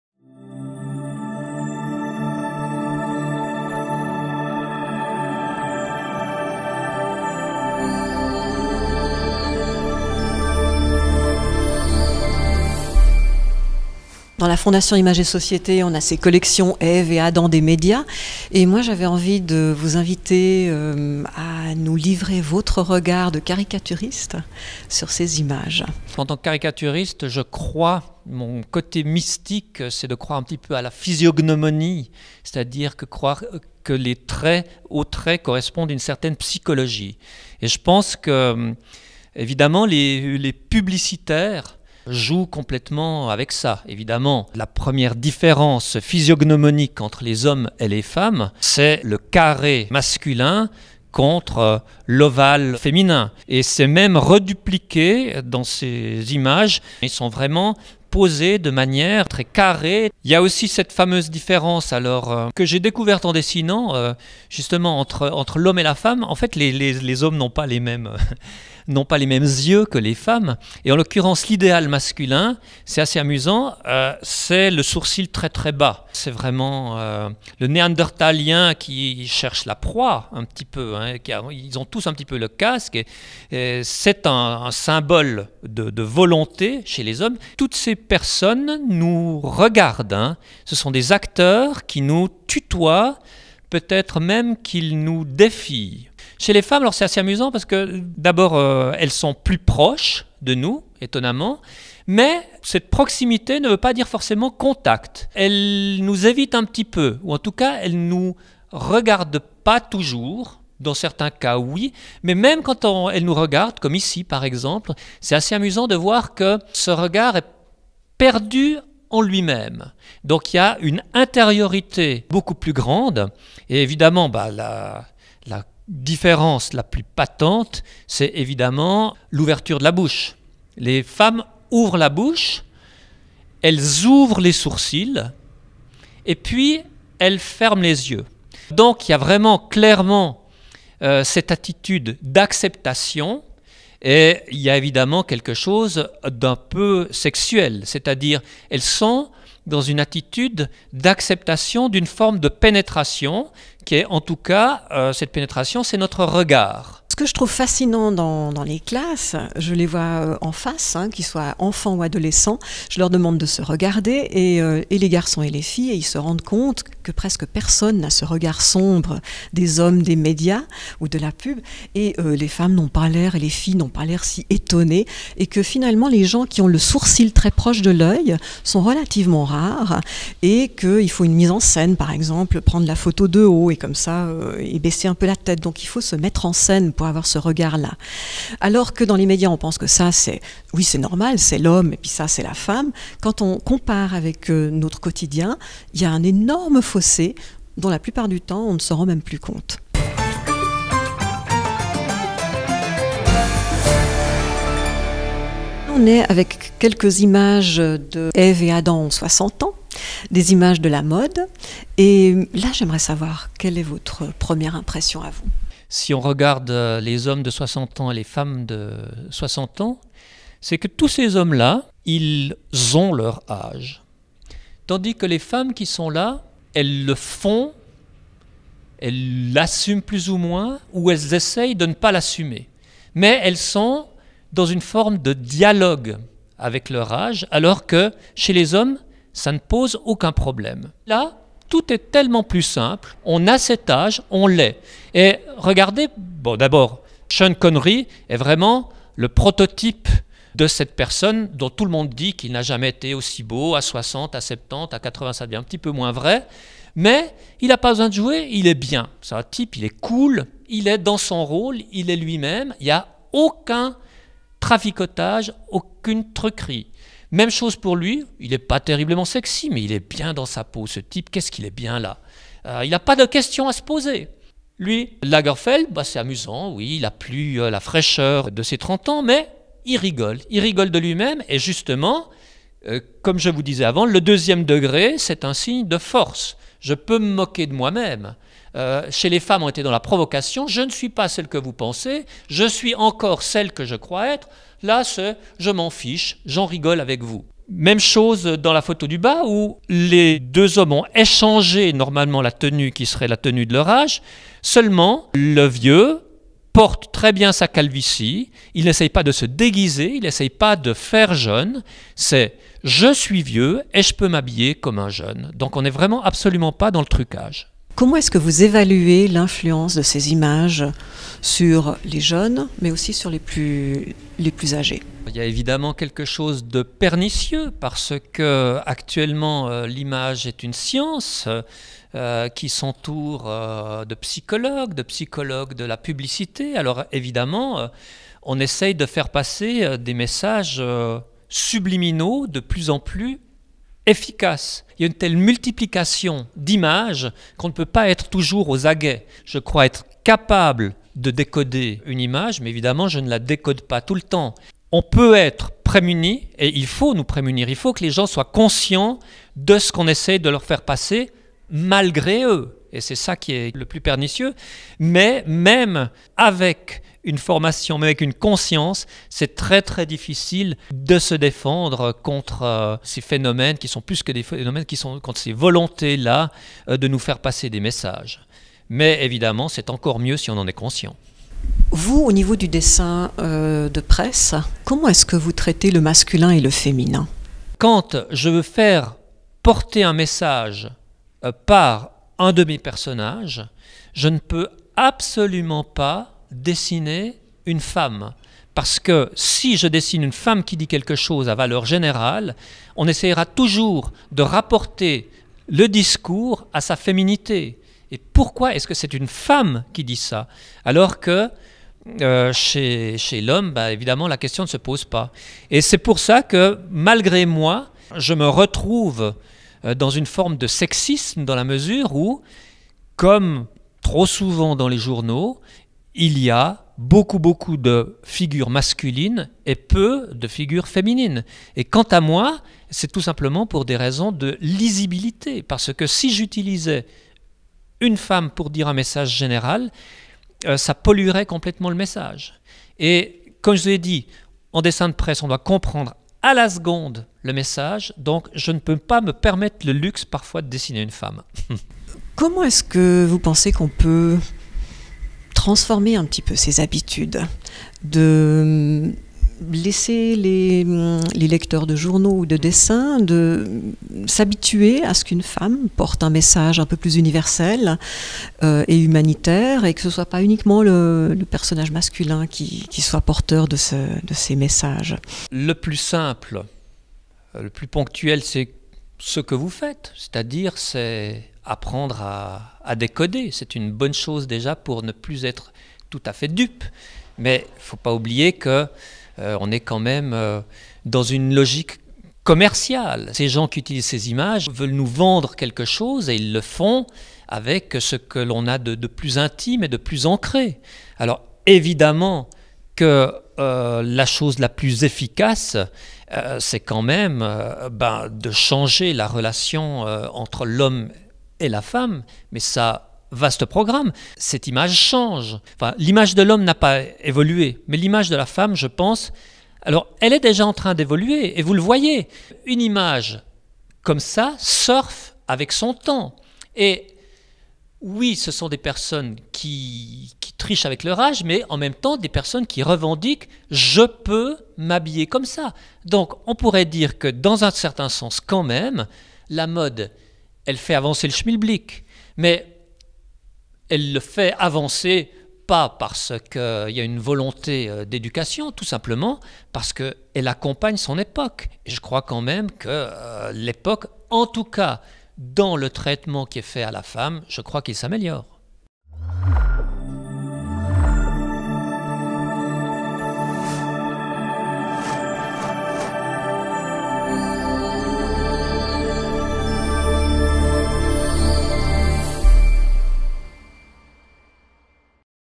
Un regard de caricaturiste sur la publicité
Comment un-e caricaturiste voit-il les clichés de la pub? Gérald Herrmann, dessinateur de presse, partage ses perspectives sur "Eve et Adam des médias", une expo-atelier de la fondation images et société.